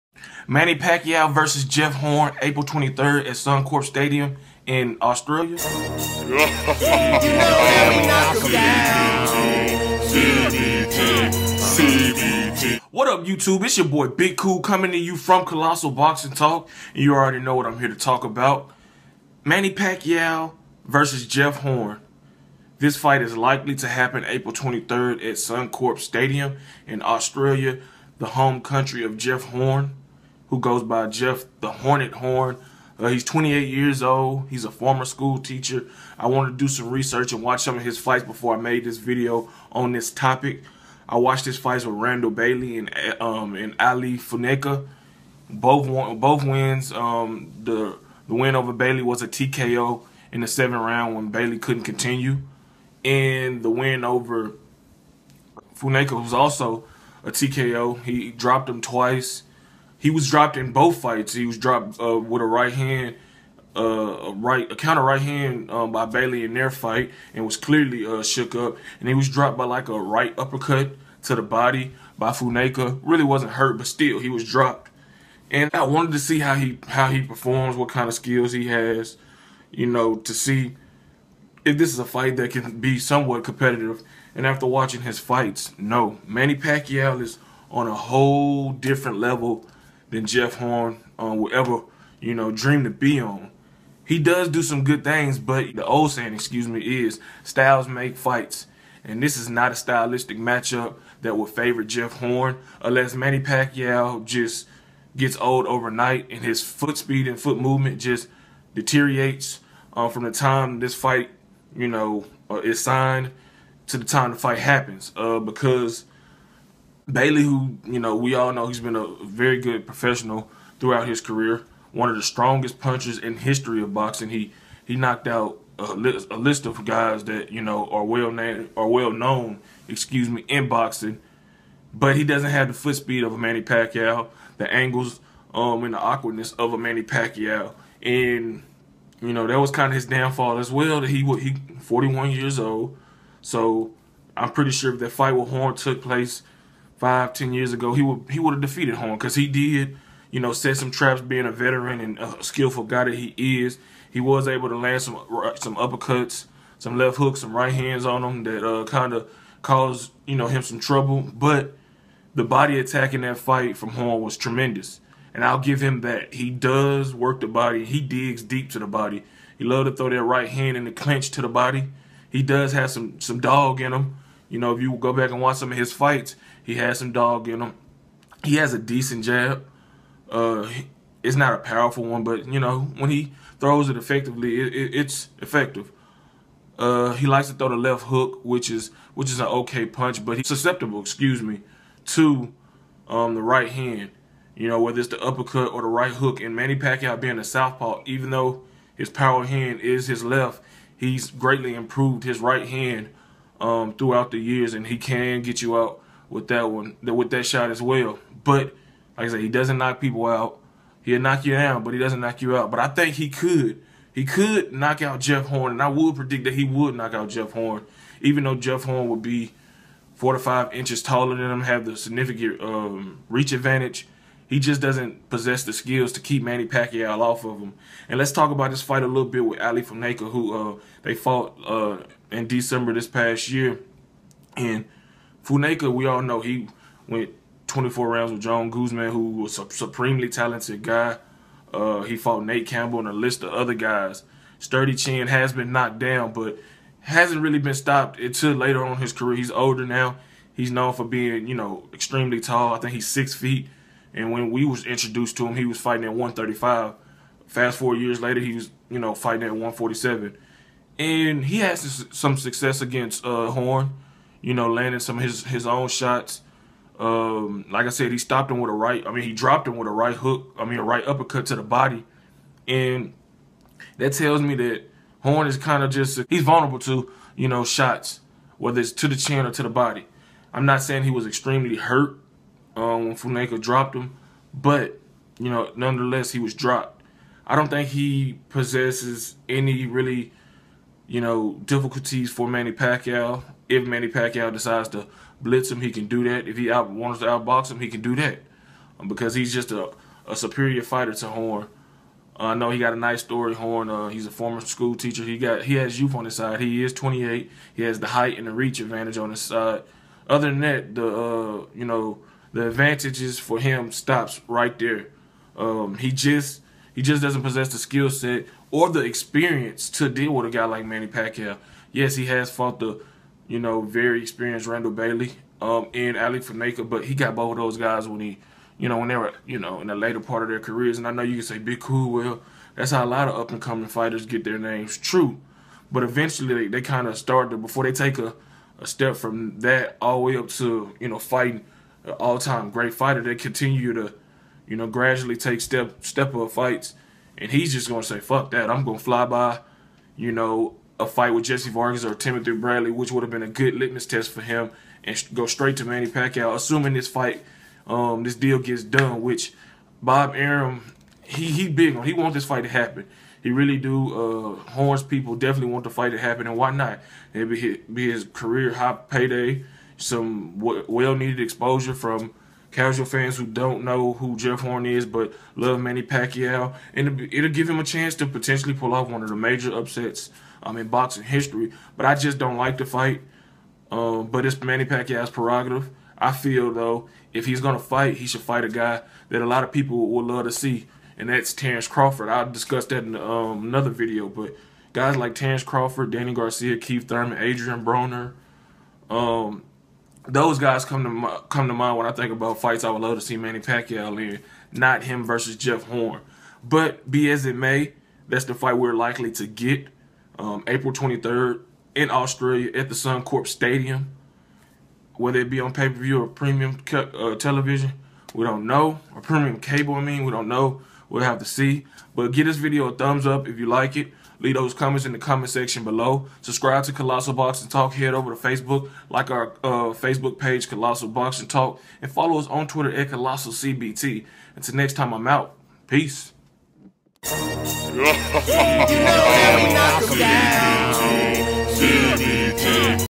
Manny Pacquiao vs Jeff Horn on April 23rd?
What up CBT fam? It's your boy Big Kool back with another video! In today's video I give you my thoughts on likelihood on Manny Pacquiao defending his WBO welterweight title against the relatively unknown Jeff Horn. Thanks